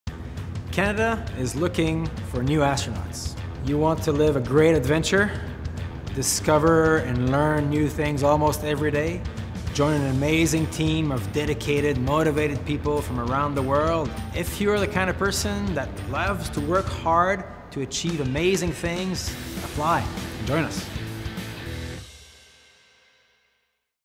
David Saint-Jacques encourages you to apply for the astronaut position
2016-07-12 - Are you keen on adventure, exploration, teamwork and learning new things? David Saint-Jacques encourages you to apply to become an astronaut. Join him as a member of the Canadian Space Agency astronaut corps! (Credit: Canadian Space Agency)